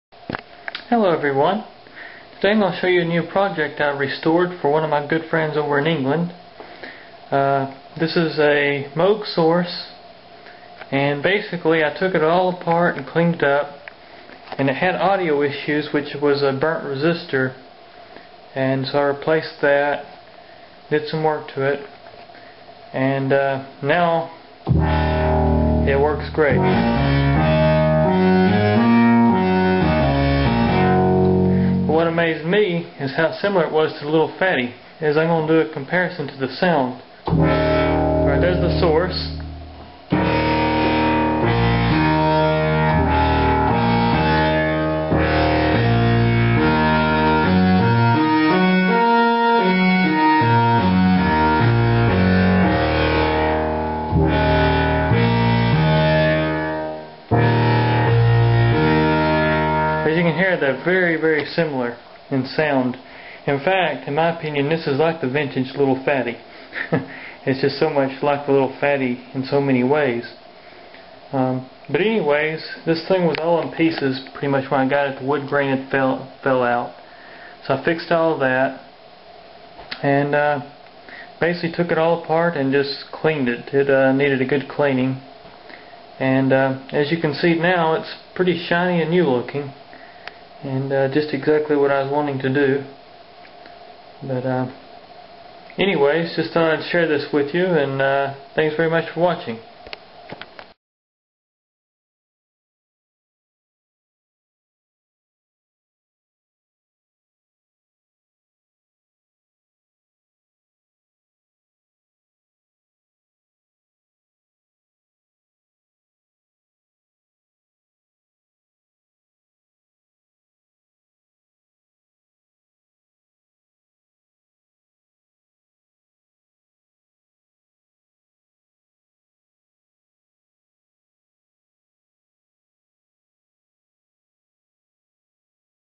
Moog the Source restoration (by Synthpro)
hello everyone, this is a very quick video I put together to show the moog source I have cleaned up for one of my friends over in England.

This synth is 100% working.

Jareth